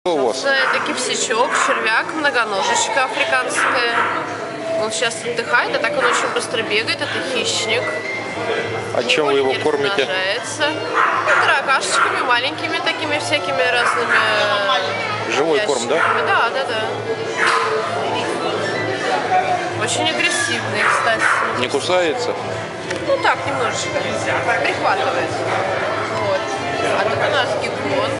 необычные домашние животные. Кивсяк Гигас Archispirostreptus gigas
Размер: до 30 см
Происхождение: Восточная, Юго-восточная Африка
Дневная температура: 25-29°C
Ночная температура: 21-23°C
Влажность: 80-90%
Размер террариума: 40x40x30 см
Субстрат: Кокосовый субстрат в смесях с землёй. Дубовые, буковые листья, как сухие, так и зелёные.
Токсичность: Крайне низкая, возможны аллергические реакции при непереносимости йода.
Темперамент: Очень спокойные, медлительные. В случае опасности сворачиваются в клубок, выделяют раздражающую жидкость с резким запахом йода.

Примечание:
Гигантских африканских многоножек или Archispirostreptus gigas, всё чаще и чаще можно встретить в домашних коллекциях экзотических животных. Африканские кивсяки вырастают до очень больших размеров и неприхотливы в содержании дома, живут от 7 до 10 лет, практически безвредны.
Защитная реакция проявляется двумя типами. Первый тип: сворачивание в клубок для защиты более мягких тканей и ног. Второй тип: выделение жидкости на основе йода, вызывающей раздражение кожи. После общения с питомцем, необходимо вымыть руки. Жидкость опасна при попадании на слизистые рта, носа, глаз. При попадании на кожу, полностью впитывается менее, чем за пять-семь часов.
Гигантские африканские многоножки имеют до 400 ног, во время линьки выростают новые сегменты, которые добавляют по четыре ноги каждый.